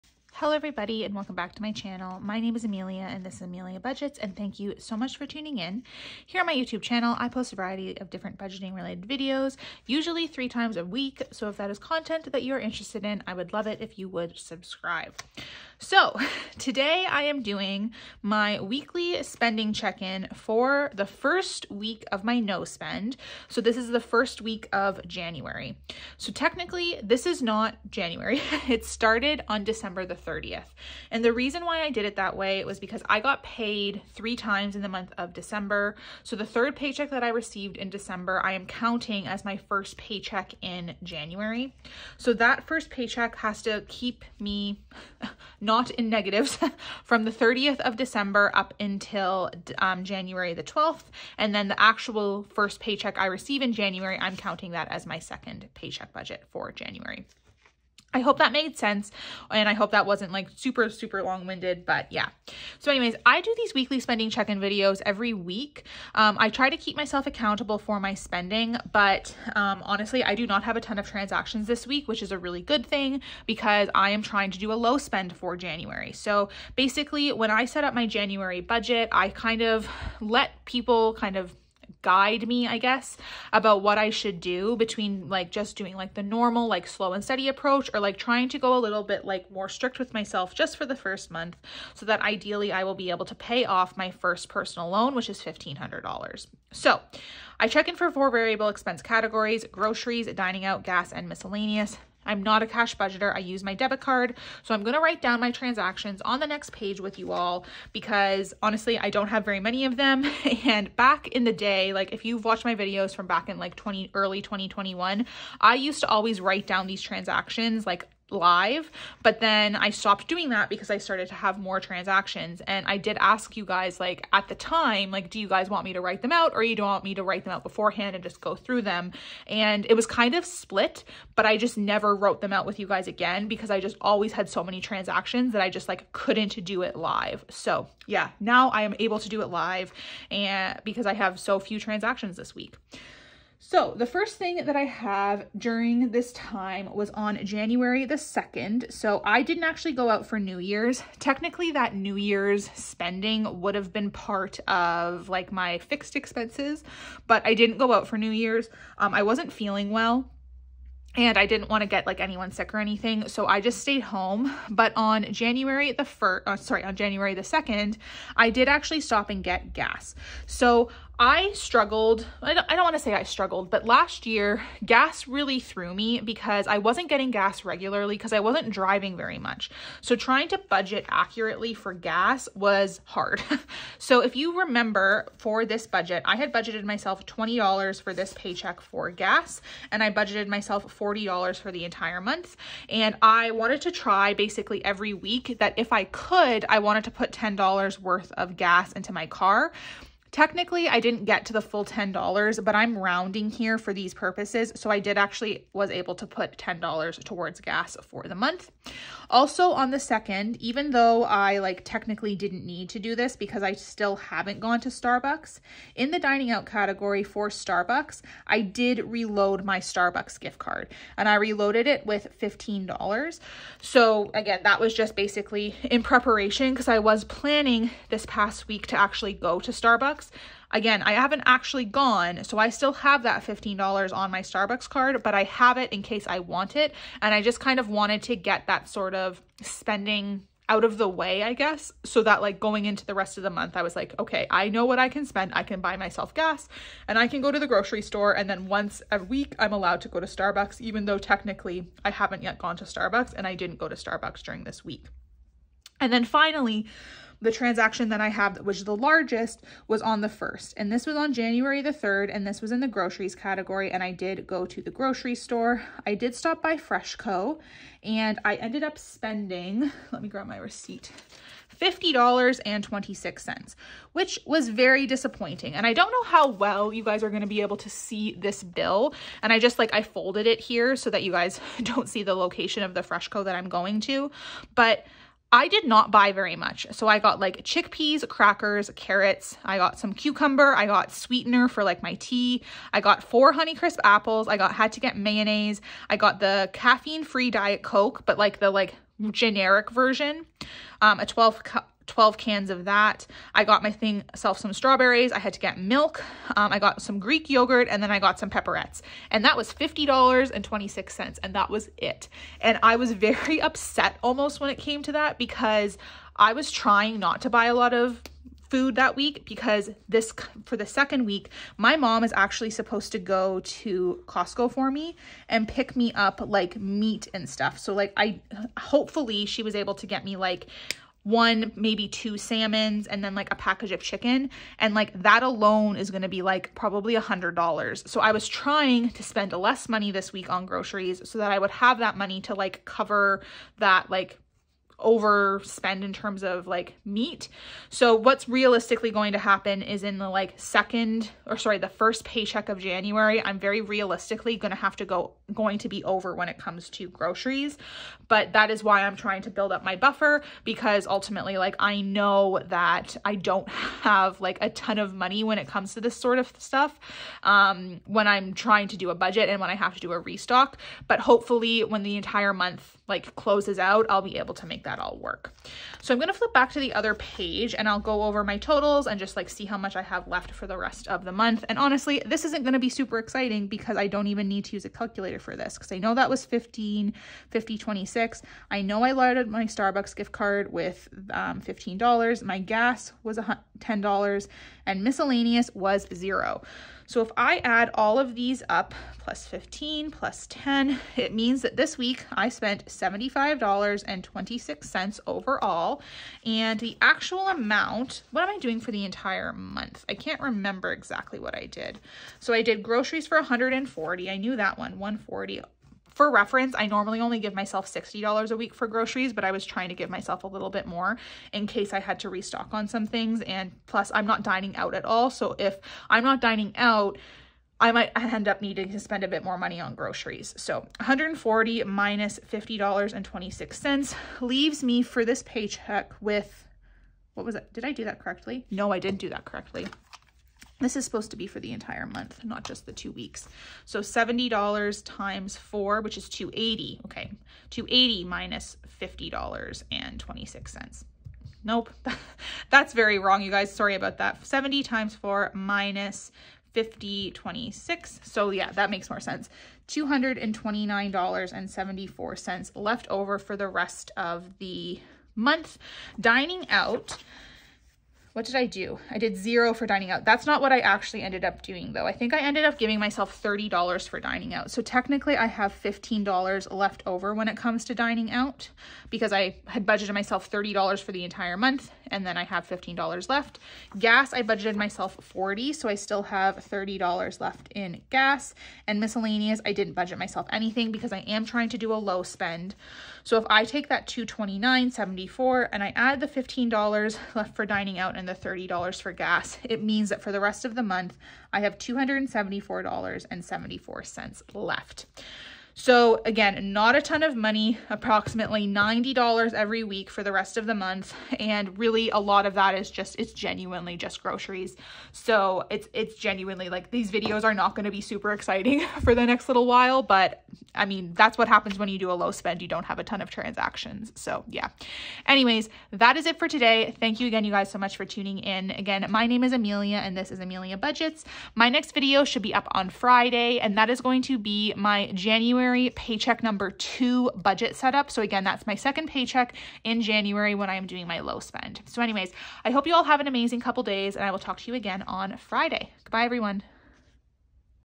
January 2023 | Week 1 Spending Check-In | First Week of my Low Spend Month ✨
My name is Amelia and I am a 27 year-old Canadian who has started budgeting to try and get out of over $25,000 of debt.